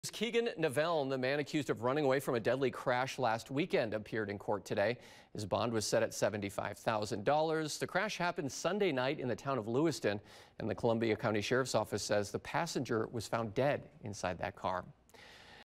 Bond set for Portage man involved in deadly crash
Authorities say Keegin Neveln, 26, is being held on $75,000 cash bond.